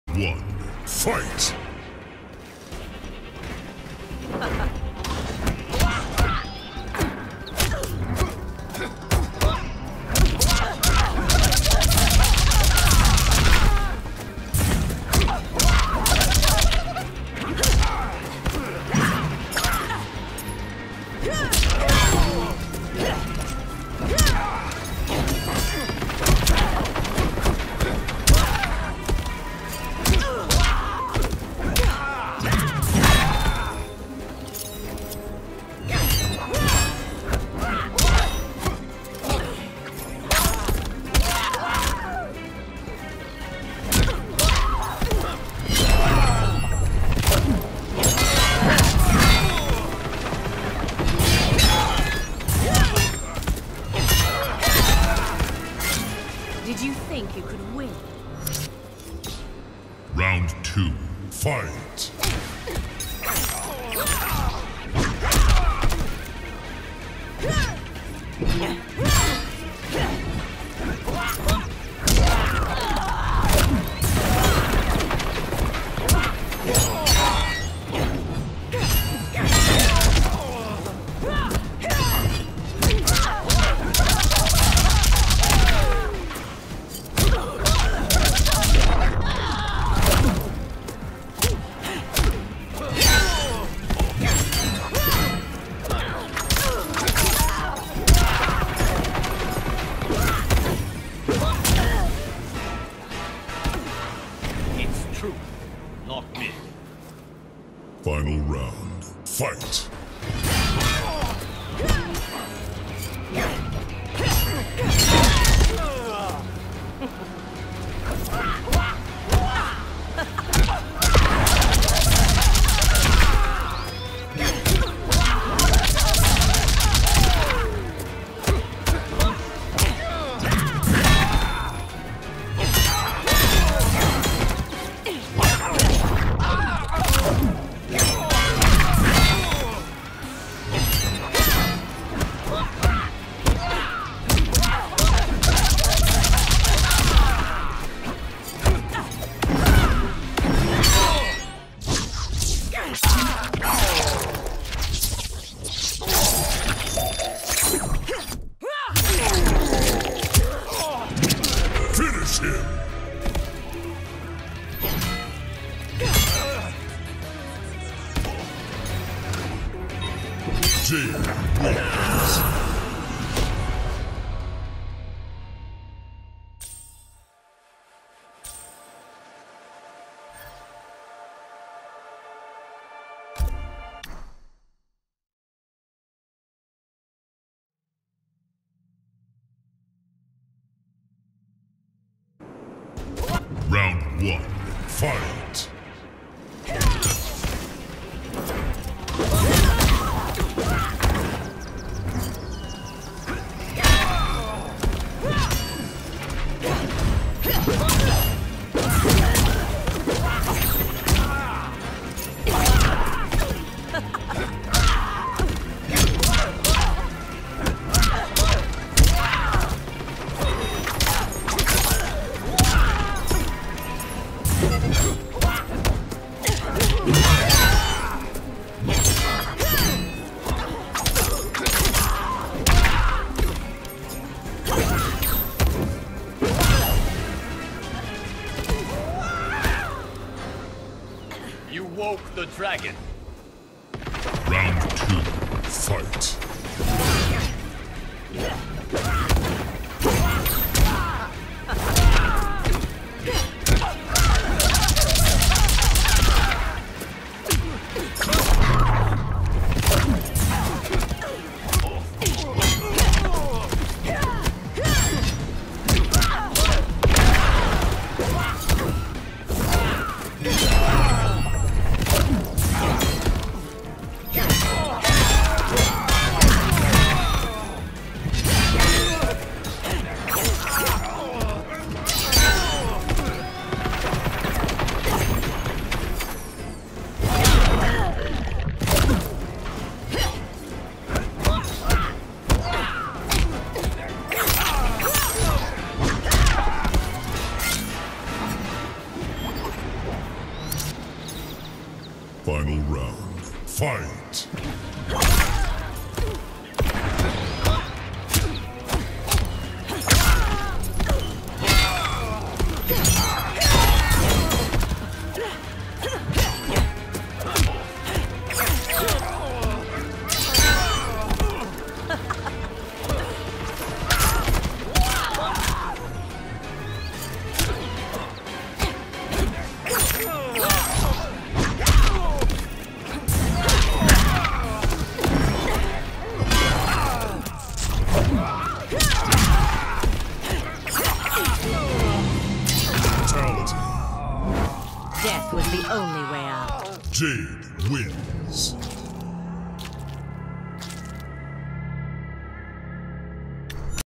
Simply Jade vs Liu
Minimum zoning, maximum fighting. If only more online matches were like this.